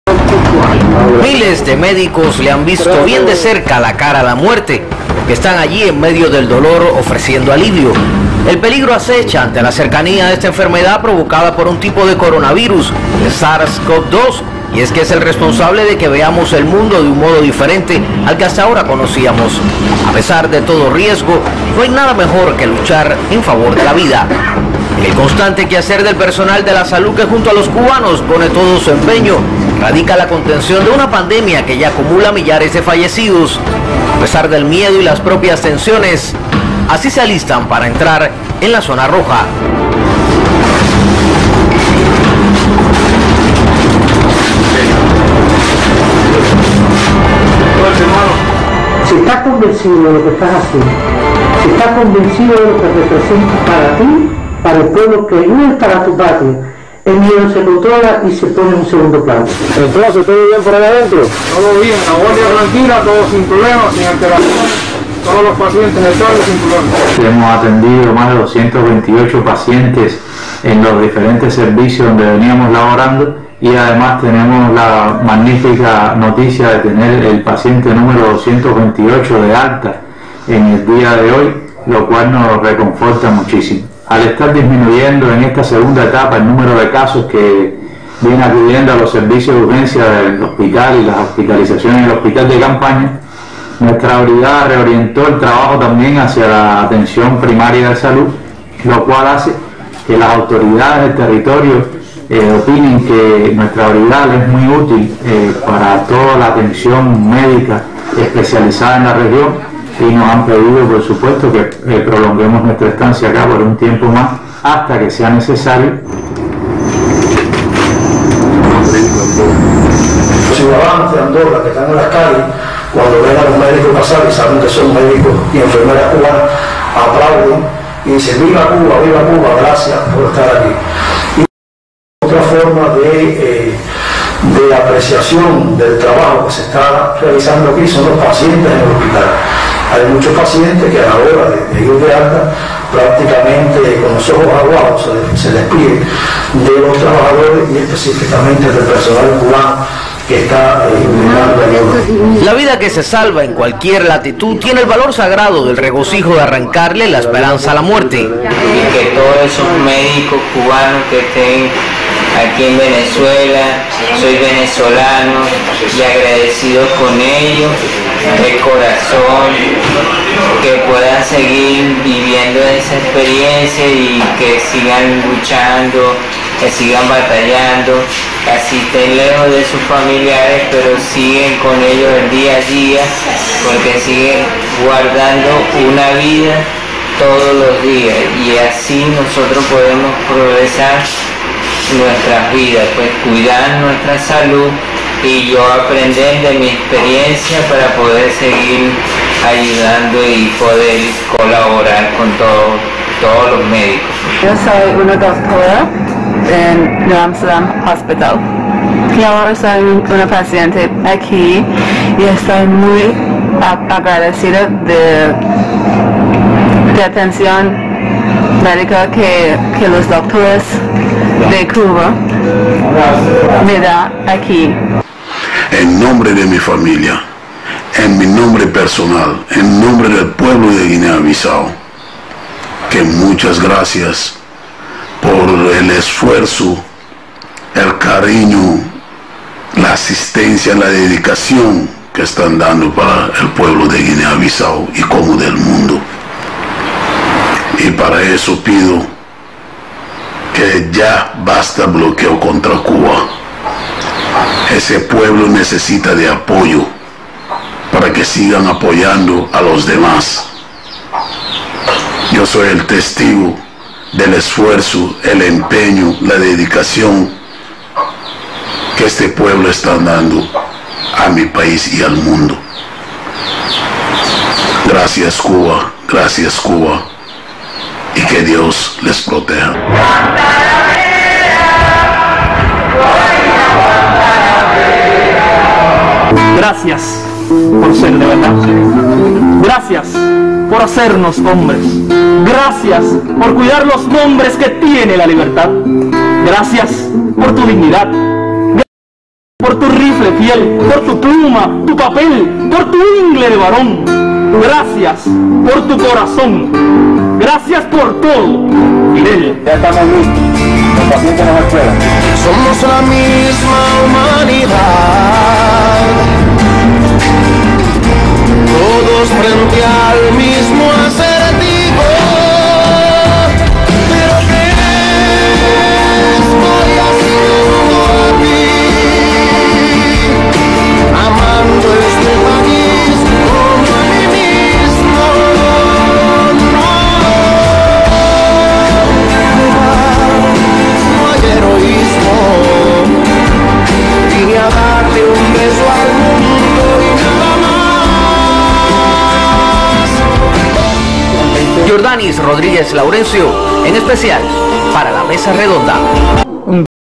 CubaSalva y combate la COVID19 por el mundo
CubaSalva y por eso buena parte personal de salud cubano se ha dispersado por el mundo para combatir la pandemia. Este material resume su labor.